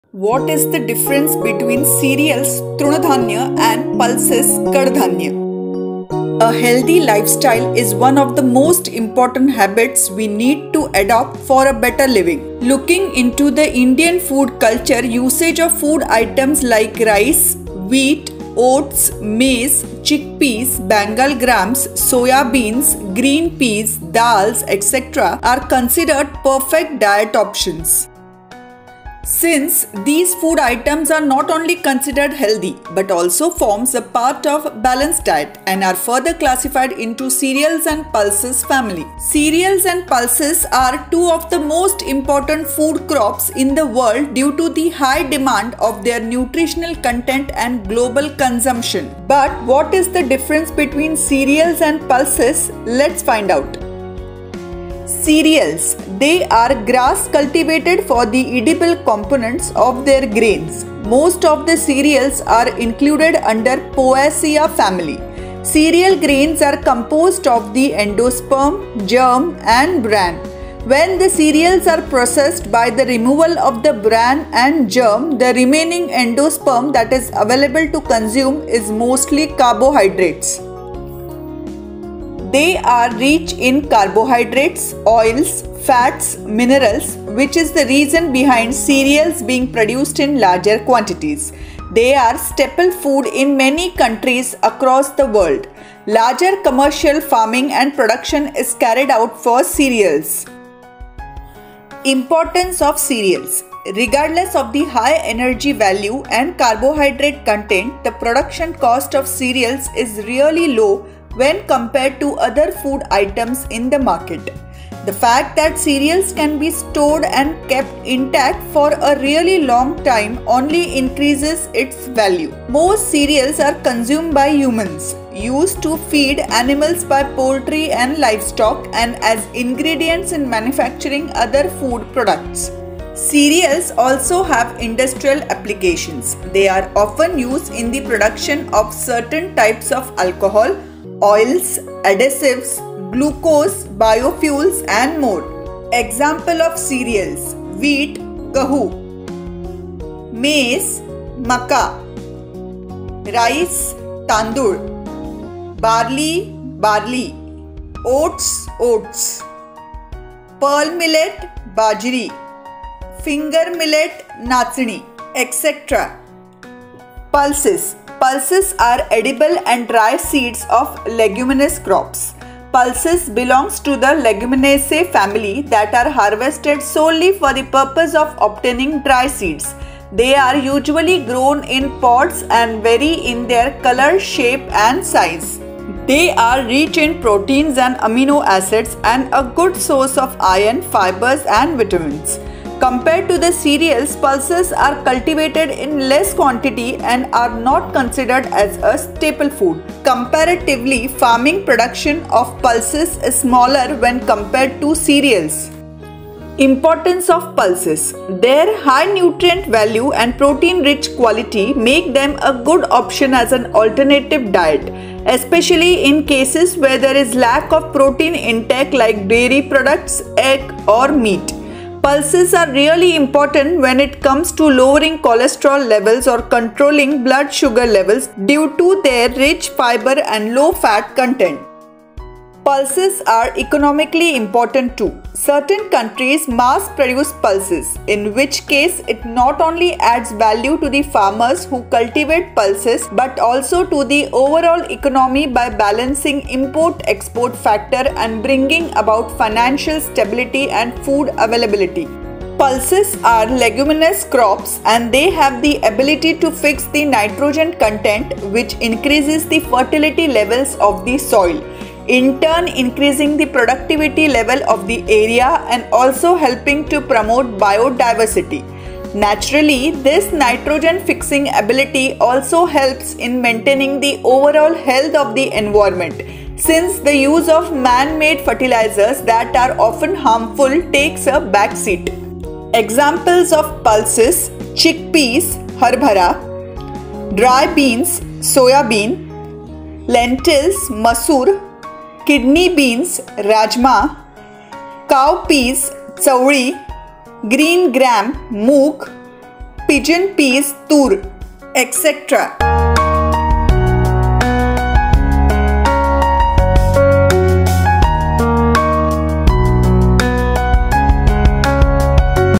What is the Difference Between Cereals (तृणधान्य) and Pulses (कडधान्य)
Topics Covered:
1. What are Cereals
2. Importance Of Cereals
3. What are Pulses
4. Importance of Pulses
5. What is the Difference Between Cereals (तृणधान्य) and Pulses (कडधान्य)

Youtube channel name: @Mahiti Khazana